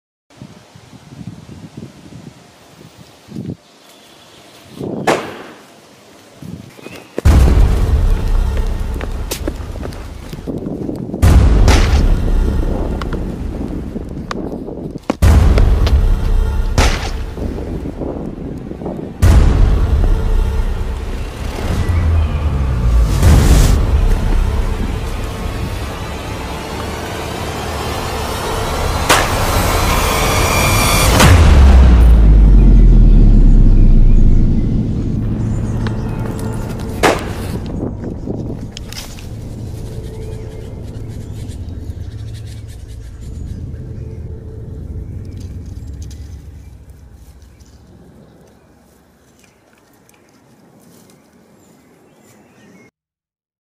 PREY - a mobile short film
I was out with the dog and had a camera with me... So why not? 😅

Once again, this is just an exercise of what can be pulled of without a computer and just an hour or so.

I tried to get some green screen assets from ProductionCrate, but either the file format didn't work out, or the keying functionality in Power Director Mobile isn't quite polished yet.